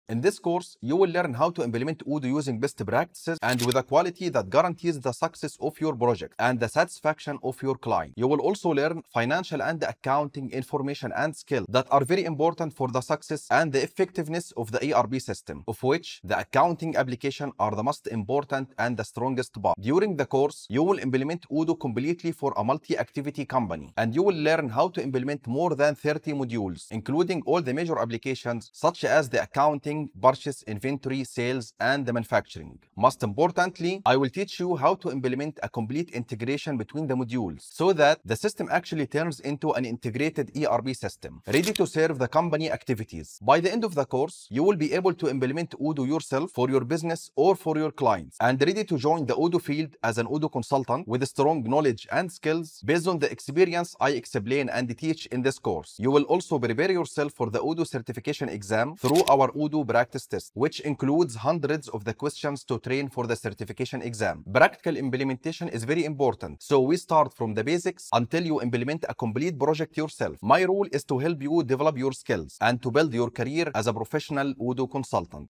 Odoo Functional Course | Learn Odoo Professionally
The Complete Odoo Professional Package: Comprehensive Functional Course + Odoo Certification Practice Test

Comprehensive understanding of Odoo's functions in the core modules and how to manage them efficiently.
Knowledge of how to implement the system to meet the needs of various companies.
Practical skills that open up new opportunities in the job market.
High-quality recorded educational content.
Simplified and straightforward explanations suitable for all levels.
Practical tests to improve your skills.

You'll also ensure your success on the Odoo certification exam with our practice test, which contains over 1,000 questions.
Practice in the same testing environment, review your answers, and retake the tests with unlimited attempts.